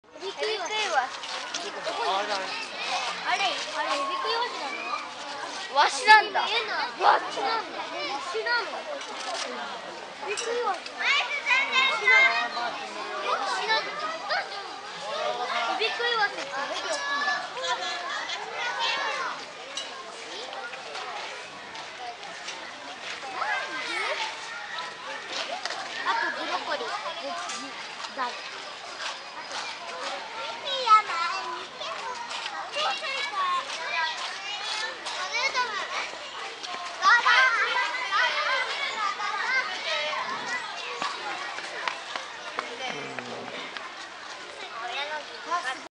上野動物園 ヘビ食いワシ
上野動物園のヘビ食い鷲。
ゲージの中を歩き回っています。